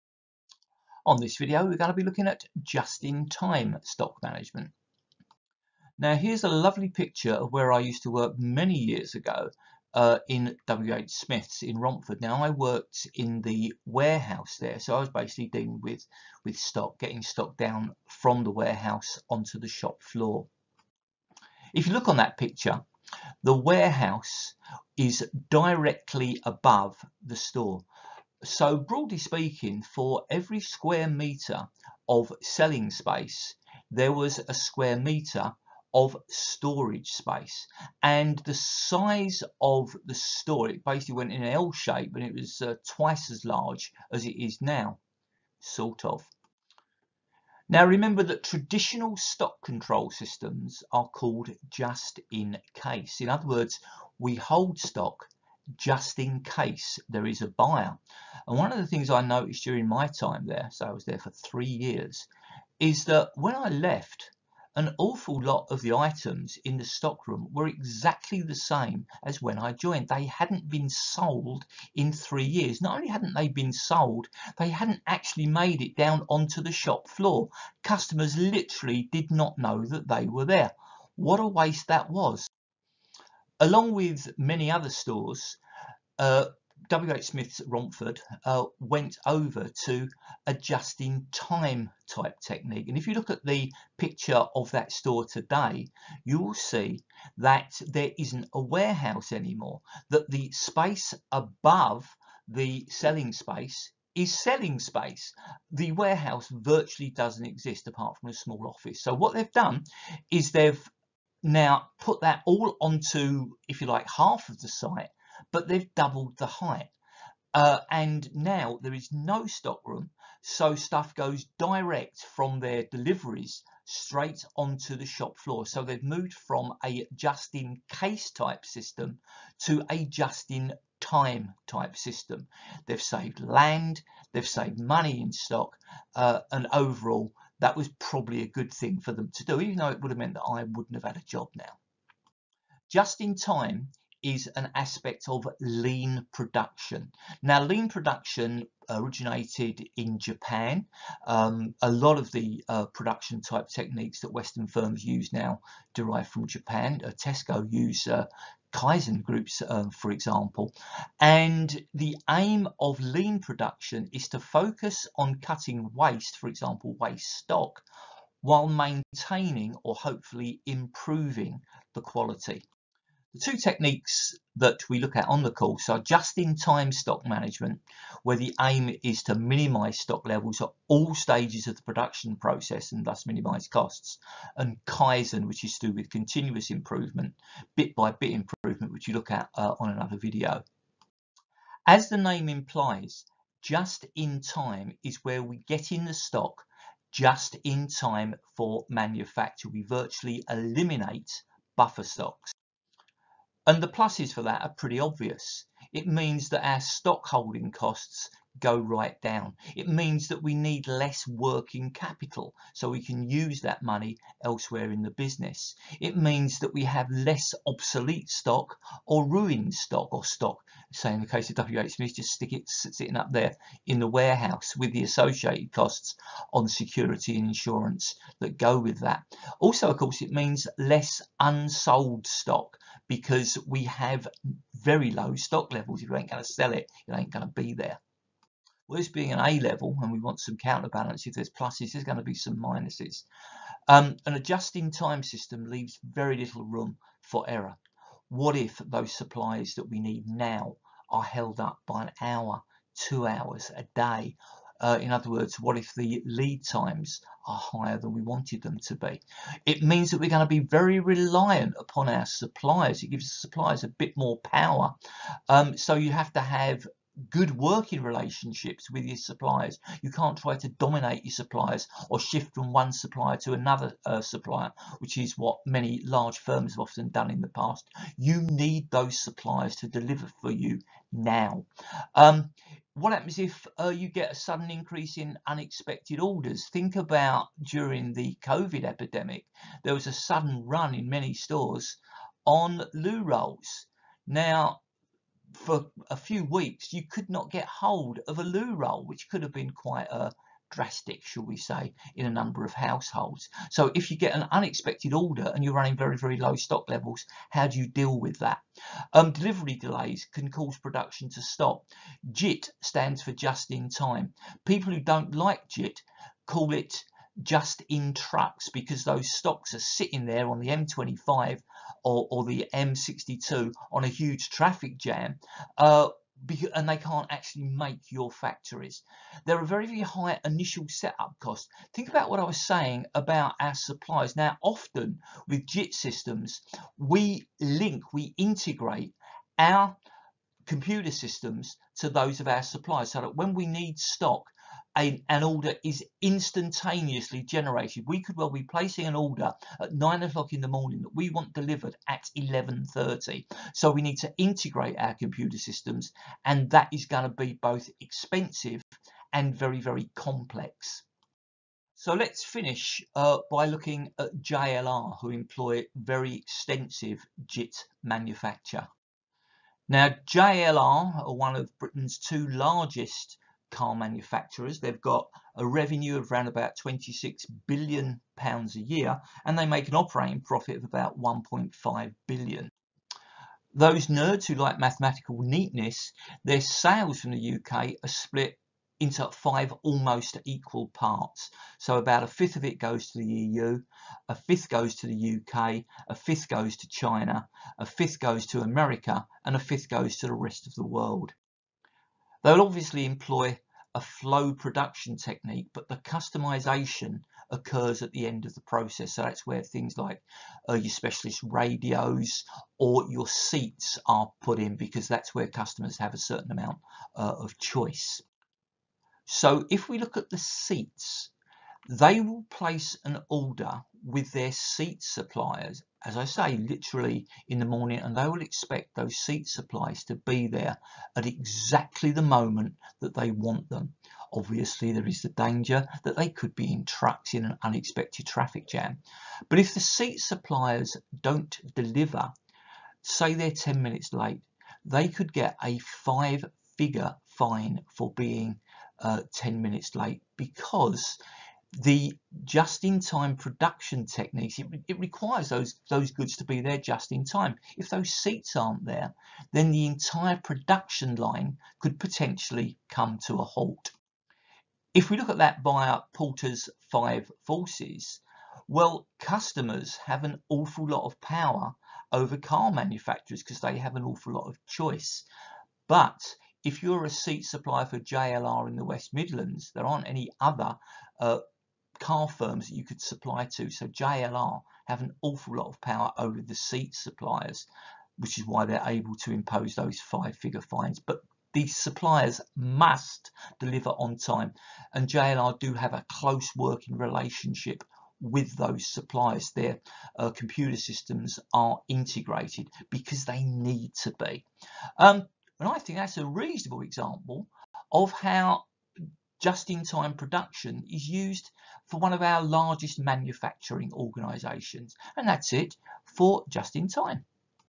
A Level Business: Lean Production
Whats and whys of lean production, including Just in Time